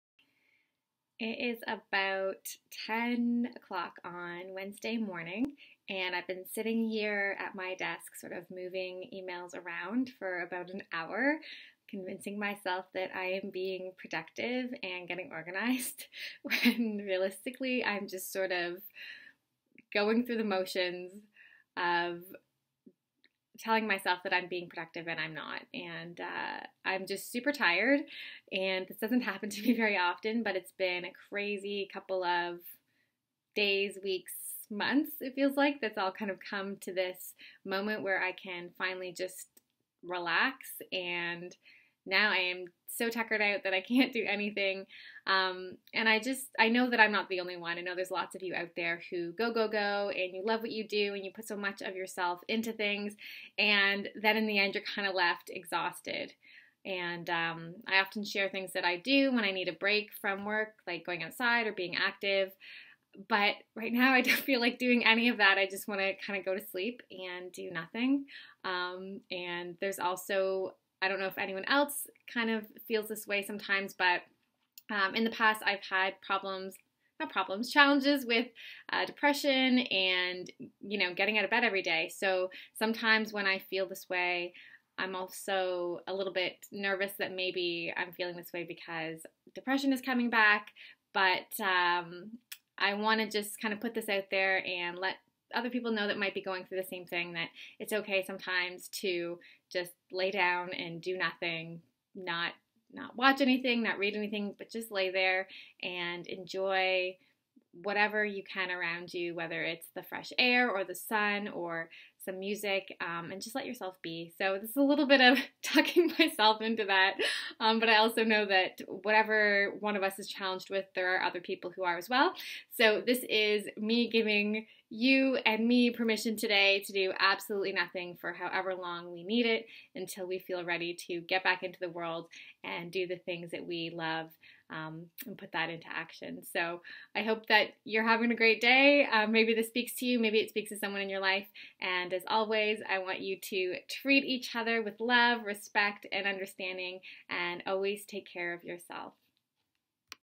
Feel Like Doing Nothing?
Let the pressure go. Give yourself a break. If you don't feel like doing anything here is permission not to. Rest and return to "action" when you're feeling ready to go again.